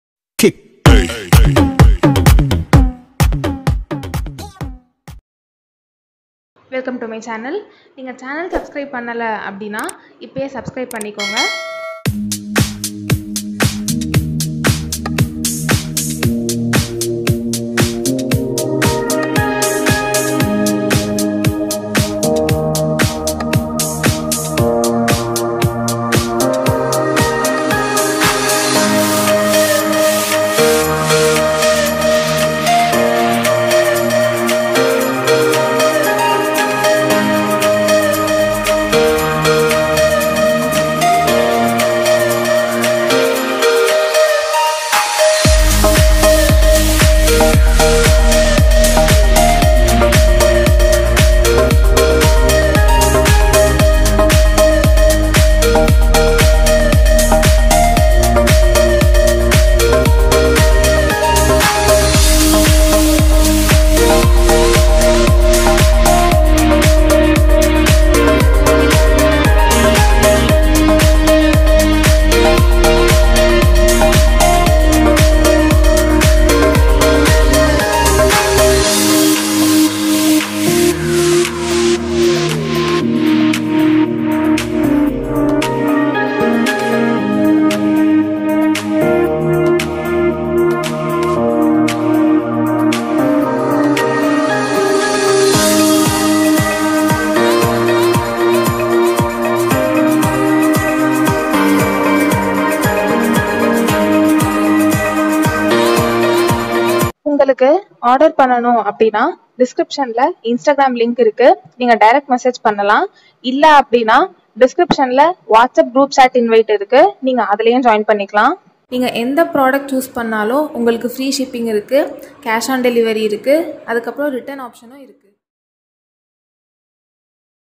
Heavy Net Sarees with Banglori Silk Blouse | At Rs.700 /- only | Diwali Collections | Part 4
Saree Fabric: Heavy Net
Blouse: Separate Blouse Piece
Blouse Fabric: Banglori Silk
Work: Embroidered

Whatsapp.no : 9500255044

*Cash on delivery / Online payment

*7days Return policy

*Original image of the product will be shown before ordering